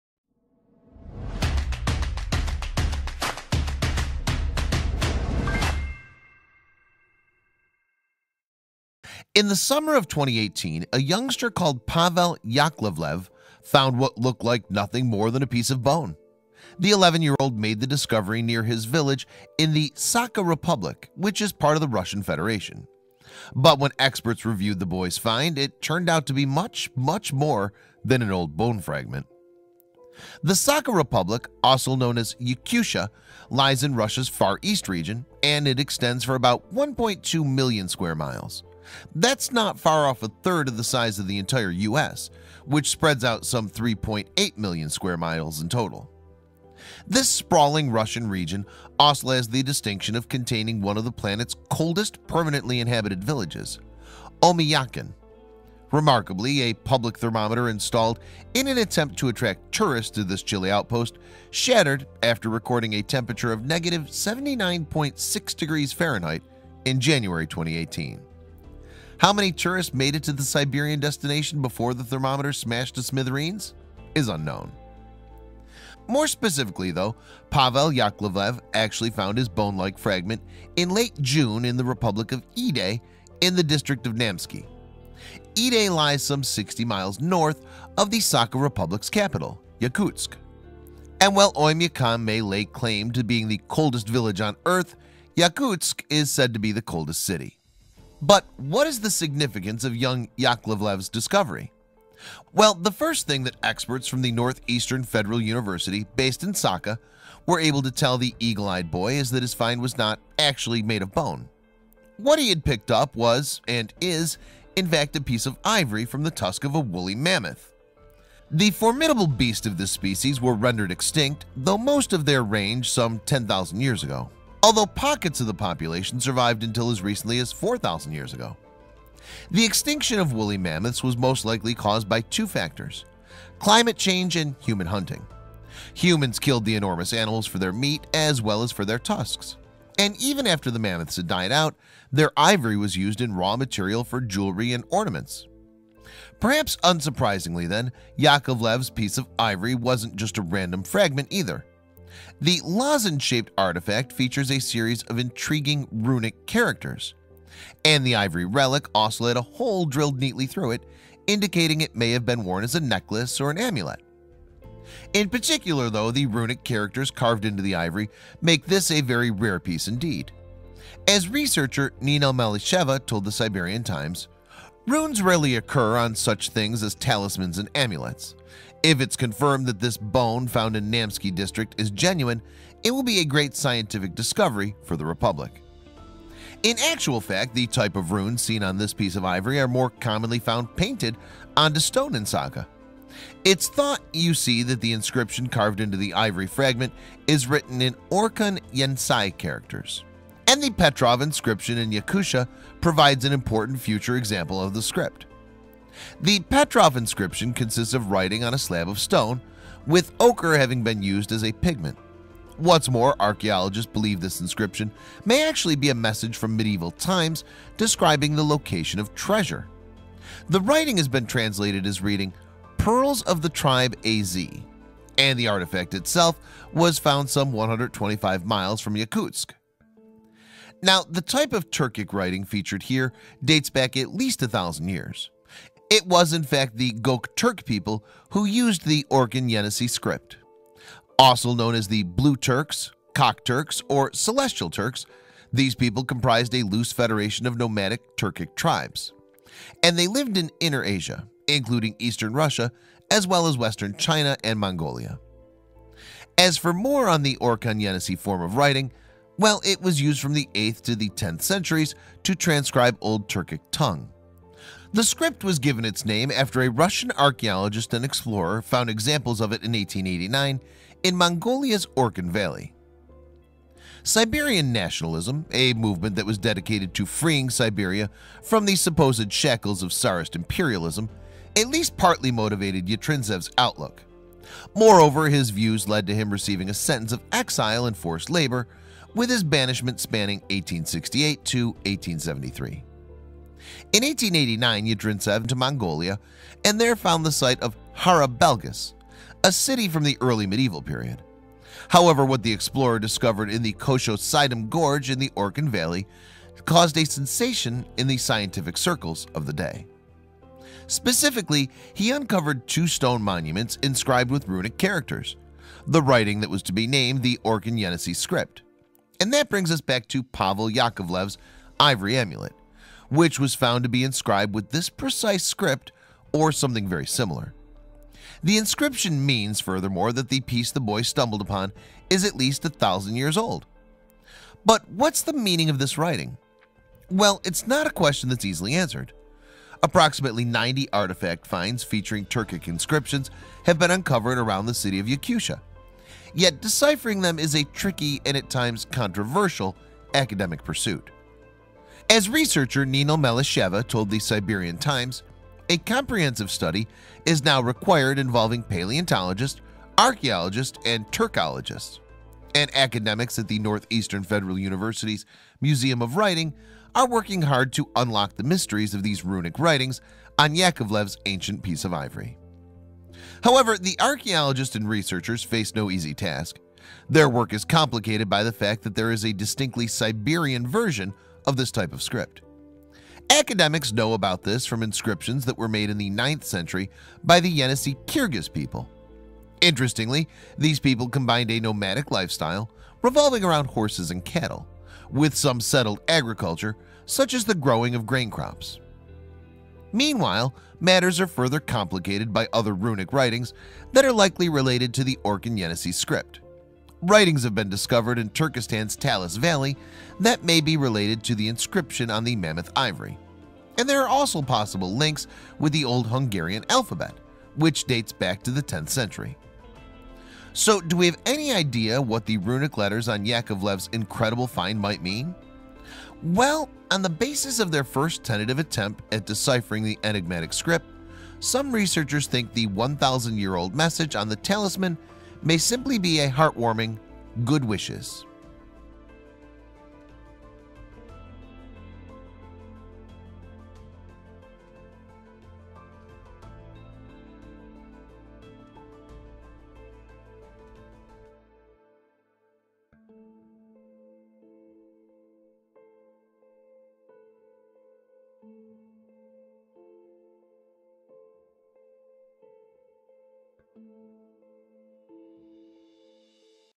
A Russian Boy Found A Fragment Of Mammoth Tusk Inscribed With A Cryptic 1,000 Year Old Message
Near his Siberian village, Russian schoolboy Pavel Yakovlev found a piece of tusk inscribed with enigmatic characters. Then experts realized that it constitutes an important archaeological discovery. In the summer of 2018, a youngster called Pavel Yakovlev found what looked like nothing more than a piece of bone. The 11-year-old made the discovery near his…

►Image credits:
Image: Flying Puffin
Image: George Rose/Getty Images
Image: NEFU via The Siberian Times
Image: Magnús H. Björnsson